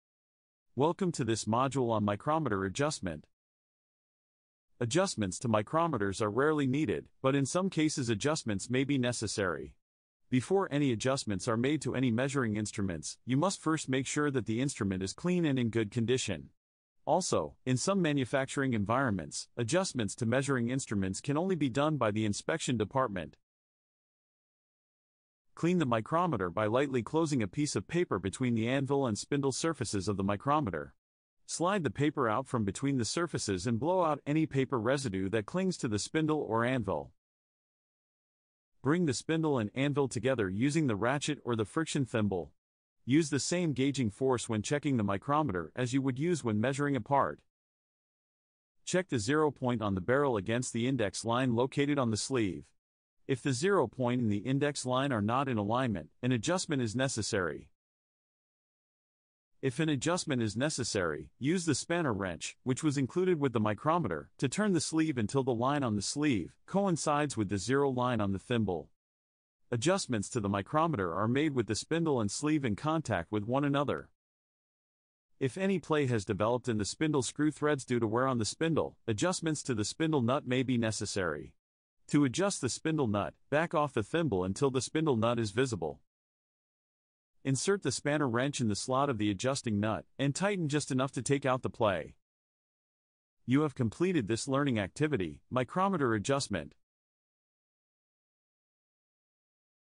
Micrometer Adjustment
Use a spanner wrench to adjust the micrometer to zero.
Credits:
    - Content Author: Kelly Curran